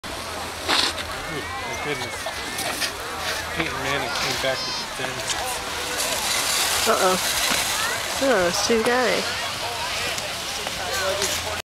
Nicole skiing fast on bunny slopes
We thought she was going to crash into the people waiting at the front of the line, but she learned how to stop pretty quickly.